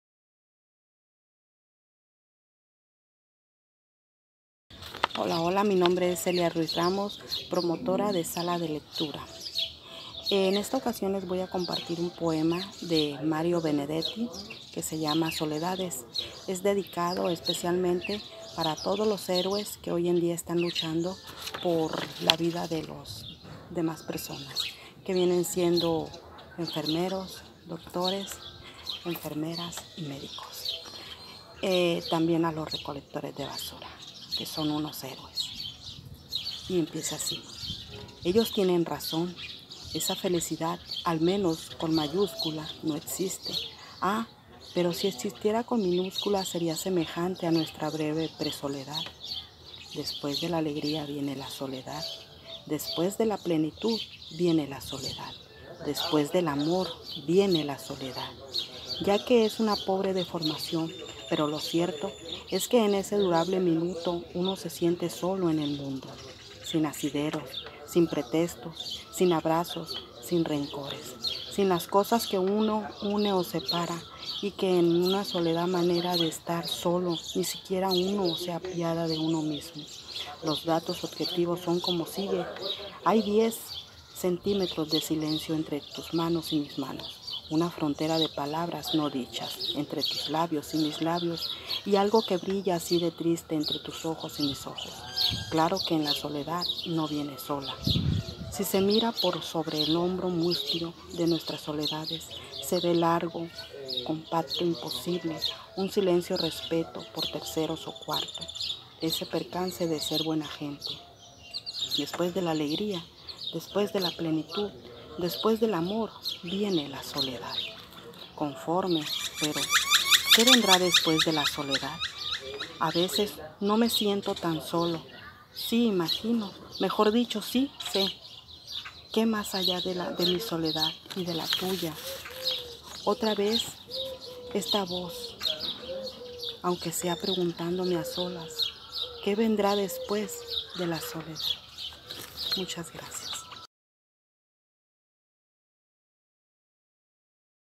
ISICDESDECASA Dedicatoria al personal médicos que atiende el COVID 19, por Celia Ruiz "Soledades".
SALAS DE LECTURA DE SINALOA

Con una dedicatoria muy especial a todo el personal médico que atiende la pandemia del COVID-19, la mediadora del Programa Salas de Lectura en Sinaloa, Celia Ruiz, del municipio de Escuinapa, nos comparte el poema "Soledades" de Mario Benedetti.